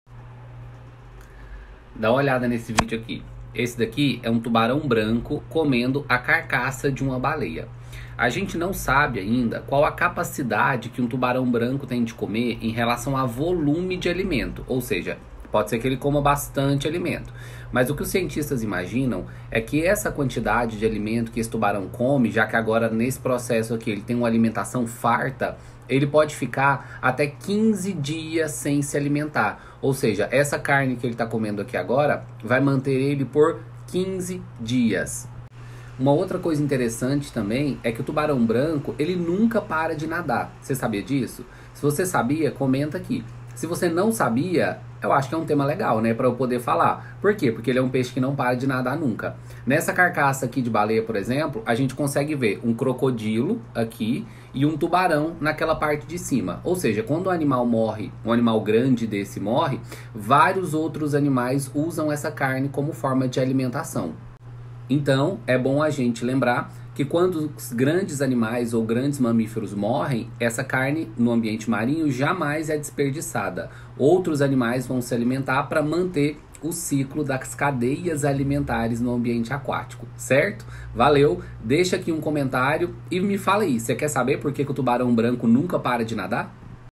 TUBARÃO comendo carcaça de BALEIA L🦈 🐋
Esse animal te assusta? 🫣😱
O tubarão-branco, tubarão-anequim, cação-anequim ou cação-branco é uma espécie de grande lamniforme que pode ser encontrada nas águas superficiais costeiras de todos os principais oceanos.
Ainda que o maior tubarão-branco registrado pesasse mais de 2.000 kg e medisse 6 m de comprimento, a maioria deles são significativamente menores. Pode ser uma surpresa, mas muitos tubarões podem alcançar um tamanho maior que os tubarões-brancos devido a isso.
De acordo com um estudo de 2014, a expectativa de vida dos tubarões-brancos é estimada em 70 anos ou mais, bem acima das estimativas anteriores, tornando-o um dos peixes cartilaginosos de vida mais longa atualmente conhecidos.
Os ataques normalmente ocorrem porque os tubarões confundem a vítima com suas presas habituais, principalmente em áreas que as águas são turvas. Os acidentes podem ocorrer ainda em decorrência da necessidade do tubarão de proteger seu território, uma atitude comum em várias espécies.